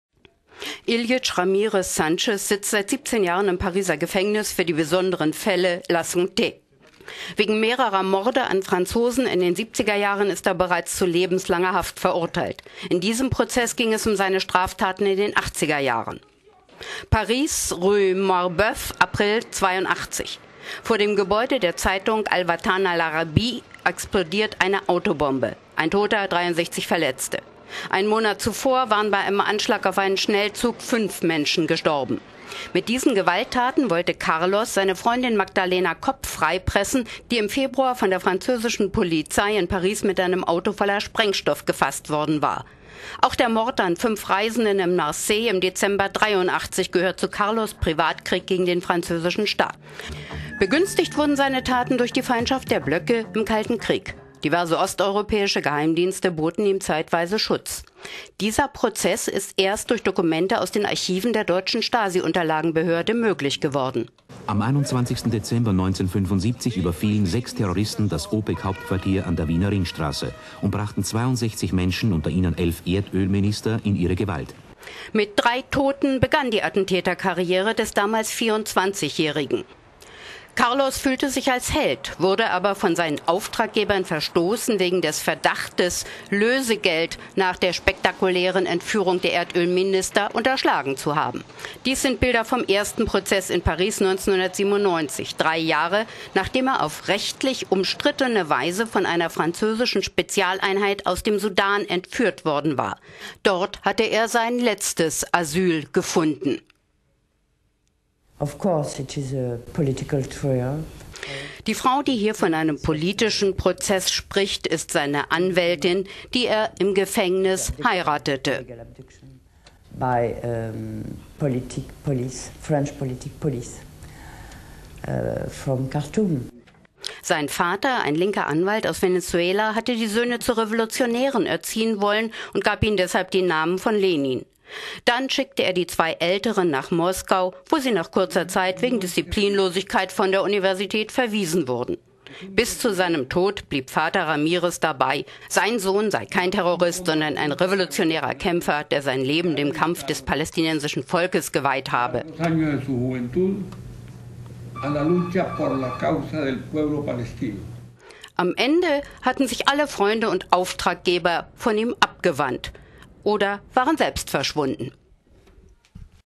Die Terroristenkarriere des Ilich Ramirez - alias "Carlos"
Ilich Ramirez Sánchez sitzt seit 17 Jahren im Pariser Gefängnis für die besonderen Fälle, La Santé. Wegen mehrerer Morde an Franzosen in den 70er Jahren ist er bereits zu lebenslanger Haft verurteilt. In diesem Prozeß ging es um Straftaten in den 80er Jahren.

Paris,  rue Marbeuf, April 1982. Vor dem Gebäude der Zeitung El Watan Al Arabi explodiert eine Autobombe. Ein Toter, 63 Verletzte.

Einen Monat zuvor waren bei einem Anschlag auf einen Schnellzug 5 Menschen gestorben.